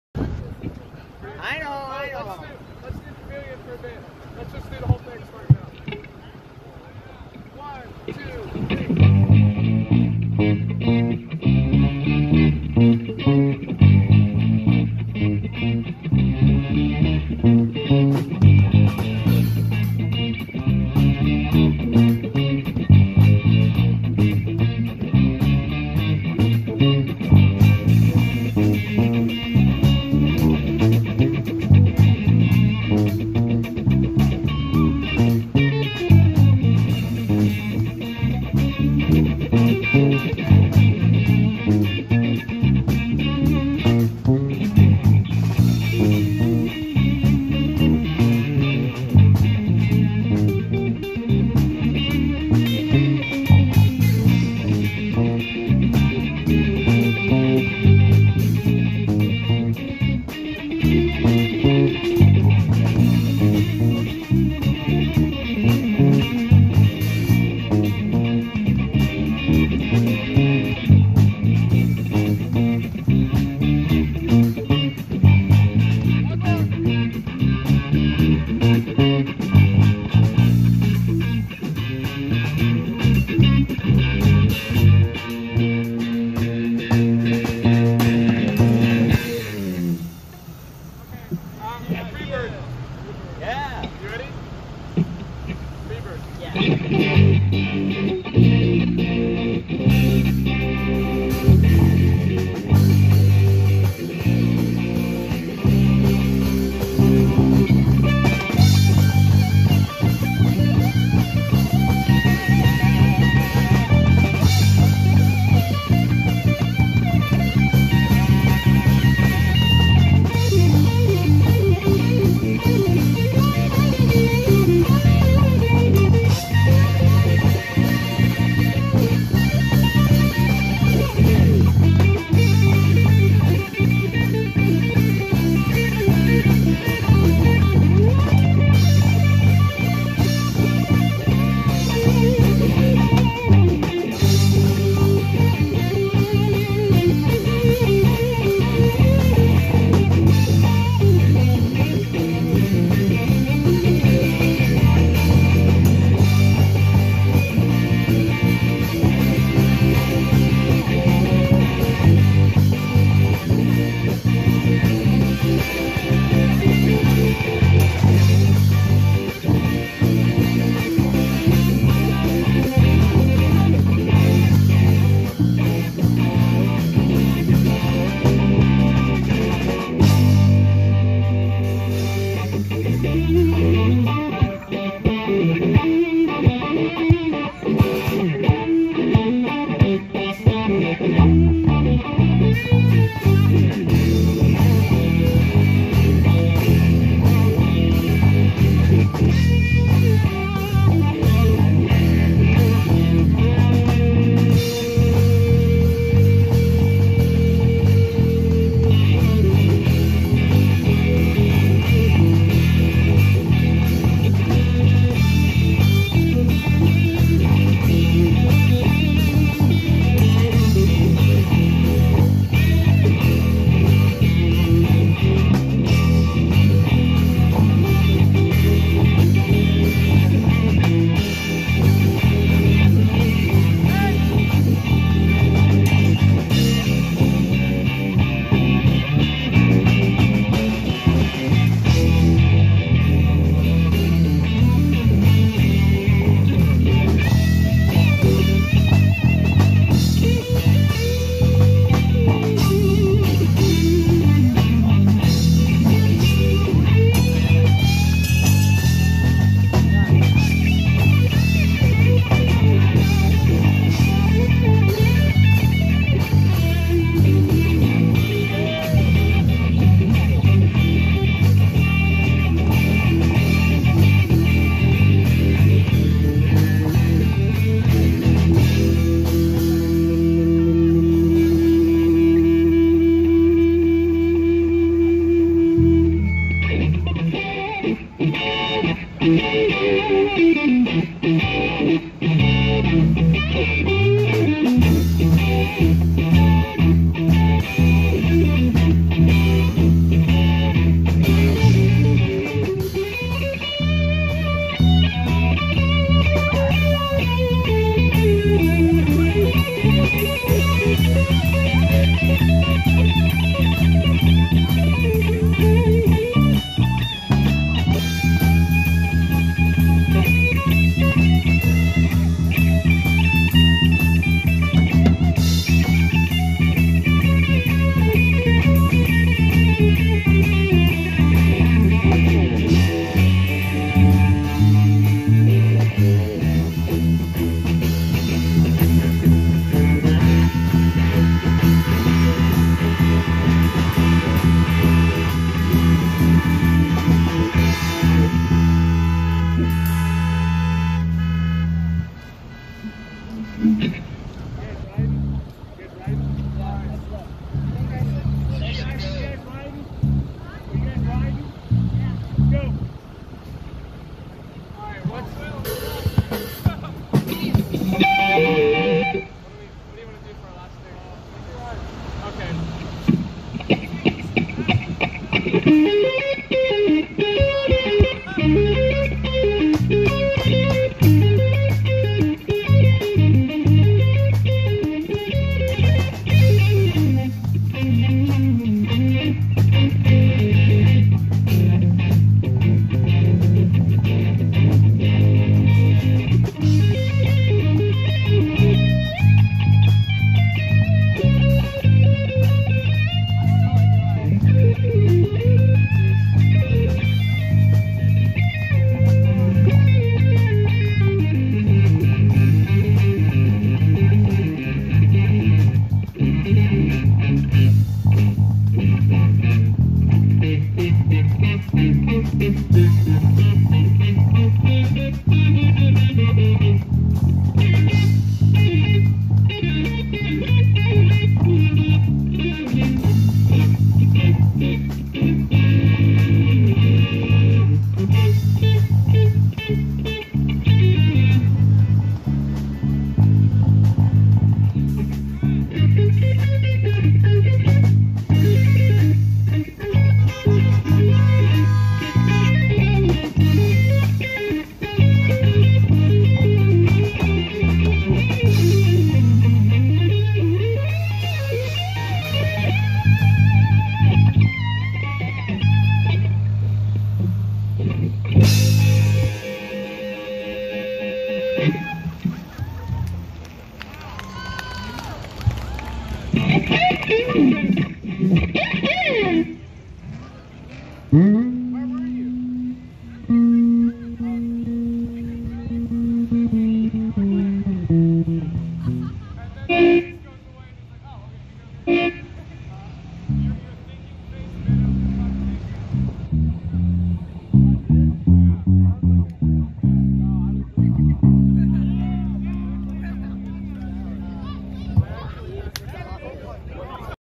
A Final School Year Jam
My friends and I finish out a school year with a bang by sending our classmates away with some music.